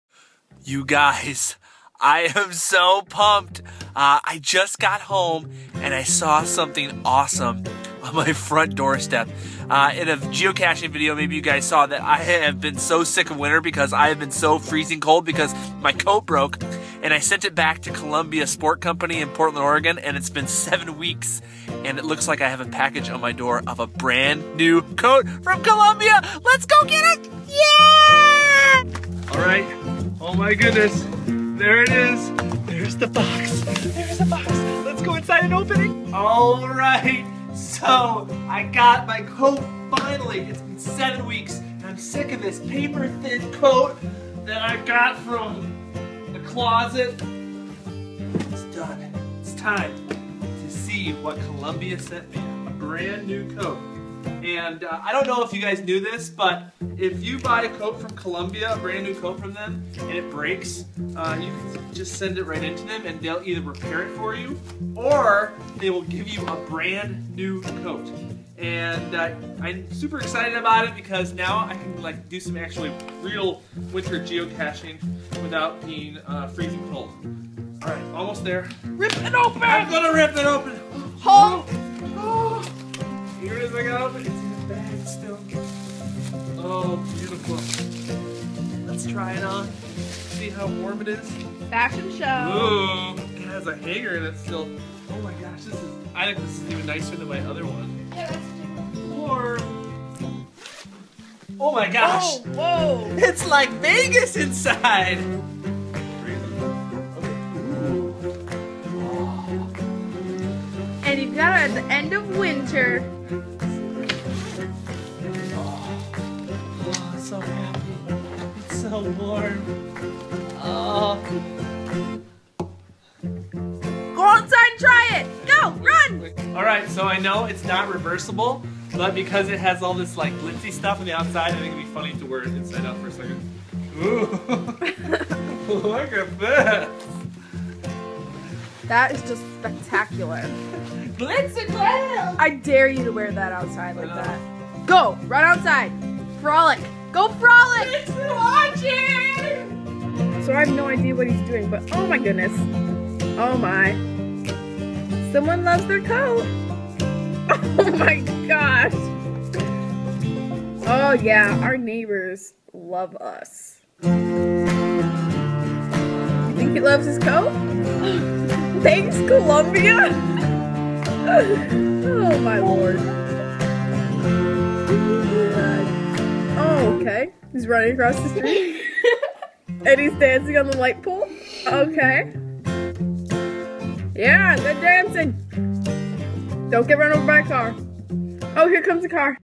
NEW COAT!
After 7 weeks with no winter coat, Columbia Sportwear sent me a brand new coat to replace my broken one. Yippee!